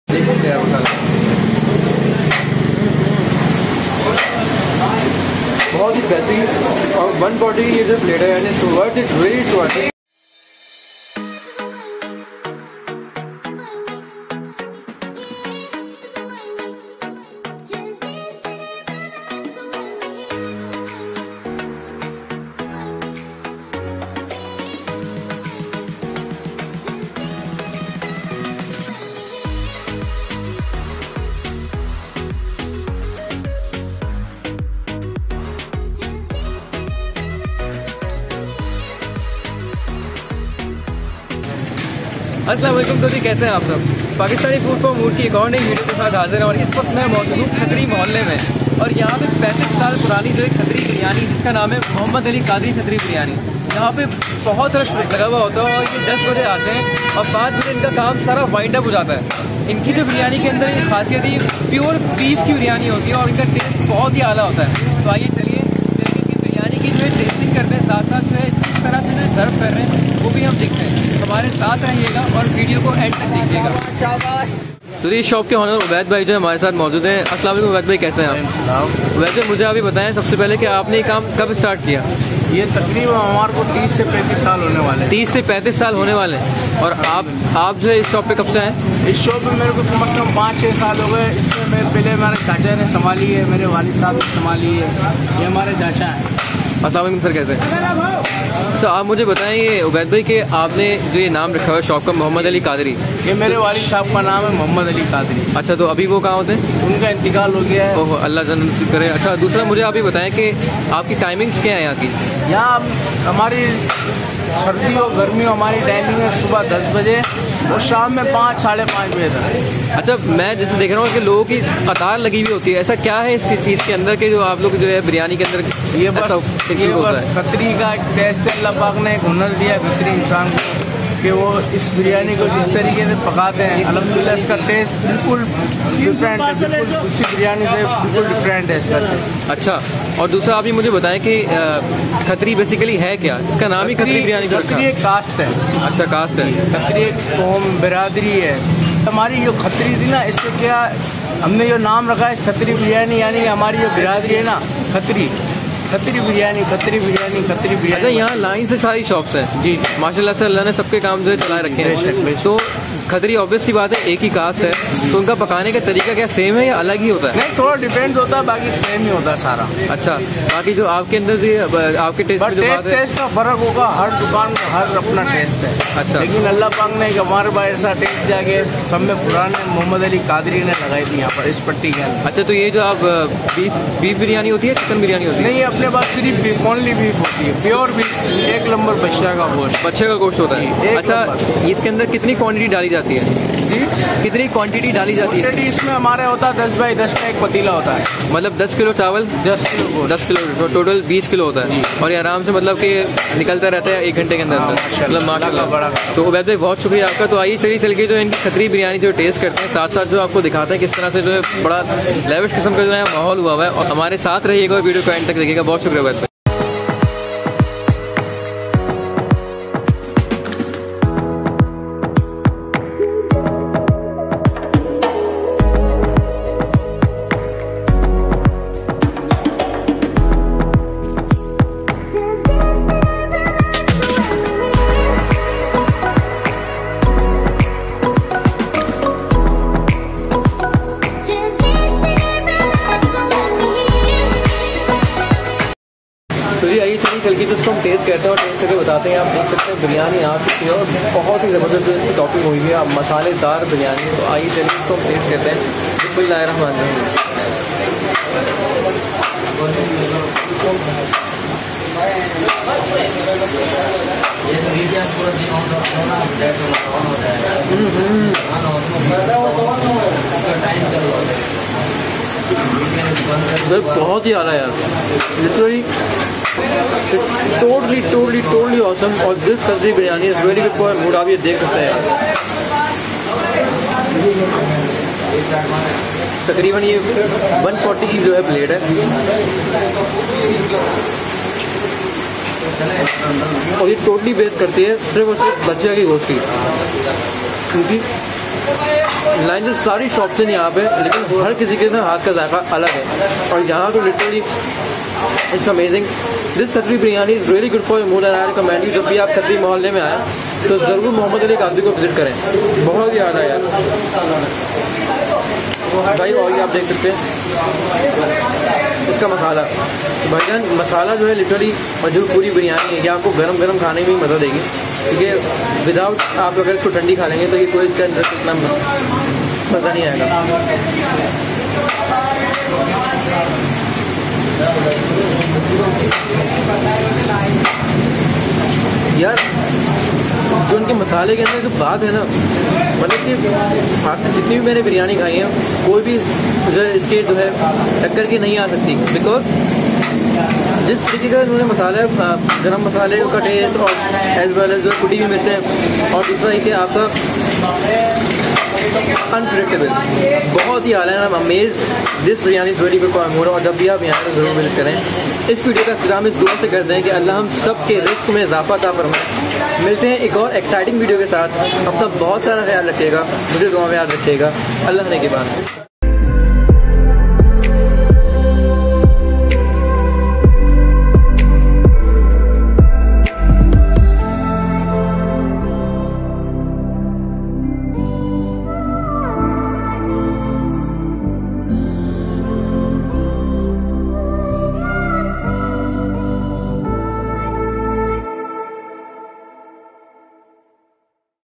Pakistani Food For Mood | Muhammad Ali Qadri Khatrri Biryani
Khatrii Mohala ki 35 sala porani " Muammad Ali Qadri Khatrri Biryani" jiski biryani bhai buht he ala hay pure beef biryani hay ap bhe jald say jald ain or taste karein... video ko complete daykheayga..

Price :

140 per plate

190 half Kg

240 special plate

Address:
Muhammad Ali Qadri Biryani